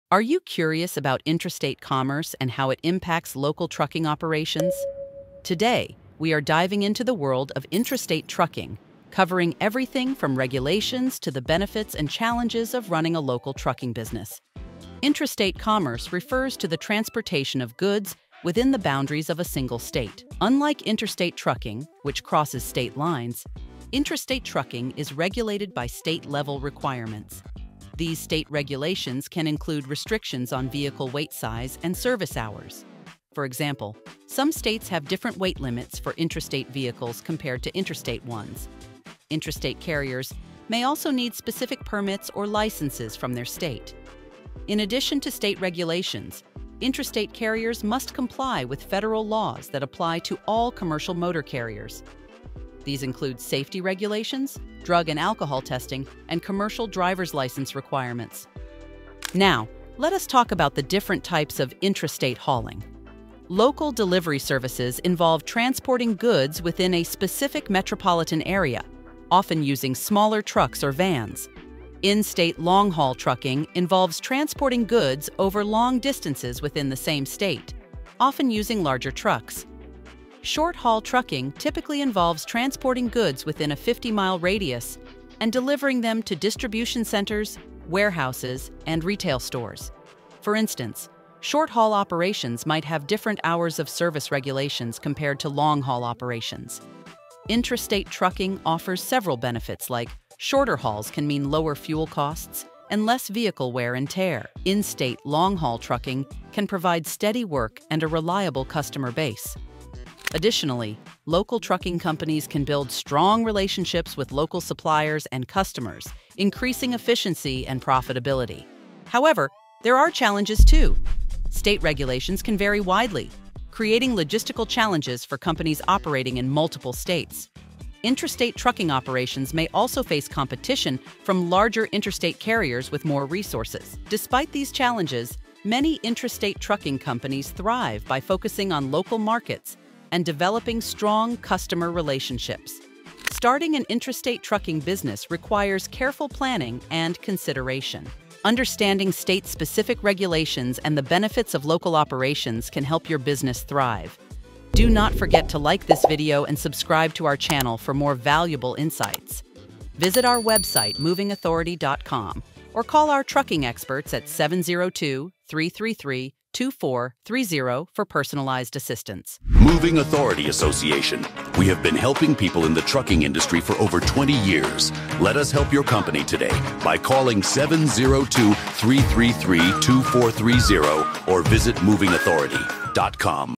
Interstate Commerce: Understanding Local Trucking Operations ⛟ 📦
Please Call PCT 9am to 5pm: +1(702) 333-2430 📱

What Is Intrastate Trucking?

Intrastate commerce refers to the transportation of goods within the boundaries of a single state. Unlike interstate trucking, which crosses state lines, intrastate trucking is regulated by state-level requirements. These state regulations can include restrictions on vehicle weight, size, and service hours. For example, some states have different weight limits for intrastate vehicles compared to interstate ones. Intrastate carriers may also need specific permits or licenses from their state.

Regulatory Compliance:
In addition to state regulations, intrastate carriers must comply with federal laws that apply to all commercial motor carriers. These include safety regulations, drug and alcohol testing, and commercial drivers' license requirements. Understanding and complying with these regulations is crucial to avoid fines and penalties. 🚚

Types of Intrastate Hauling:

Local Delivery Services: This involves transporting goods within a specific metropolitan area, often using smaller trucks or vans.
In-State Long Haul Trucking: This involves transporting goods over long distances within the same state, typically using larger trucks.
Short Haul Trucking: This usually involves transporting goods within a fifty-mile radius and delivering them to distribution centers, warehouses, and retail stores.
Each type of intrastate hauling may have specific regulations and requirements. For instance, short-haul operations might have different hours of service regulations compared to long-haul operations.

Benefits of Intrastate Trucking:

Lower Costs: Shorter hauls can mean lower fuel costs and less vehicle wear and tear.
Steady Work: In-state long-haul trucking can provide steady work and a reliable customer base.
Strong Relationships: Local trucking companies can build strong relationships with local suppliers and customers, increasing efficiency and profitability.
Challenges of Intrastate Trucking:

Varied State Regulations: State regulations can vary widely, creating logistical challenges for companies operating in multiple states.
Competition: Intrastate trucking operations may also face competition from larger interstate carriers with more resources.
Despite these challenges, many intrastate trucking companies thrive by focusing on local markets and developing strong customer relationships.

Starting an Intrastate Trucking Business:
Starting an intrastate trucking business requires careful planning and consideration. 🚛 First, obtain the necessary licenses and permits for your state. This varies depending on the type of hauling you plan to do. Next, invest in the right vehicles and equipment for your operations. Hire and train qualified drivers and staff, ensuring they are familiar with your state's routes and regulations.

Building a Customer Base:
Building a customer base is also crucial. Network with local companies and establish partnerships to secure regular hauling jobs. By carefully considering these factors and developing a solid plan, you can set up your intrastate trucking business for success.

Learn More and Get Started:
Intrastate trucking offers unique opportunities and challenges. Understanding state-specific regulations and the benefits of local operations can help your business thrive. 🚛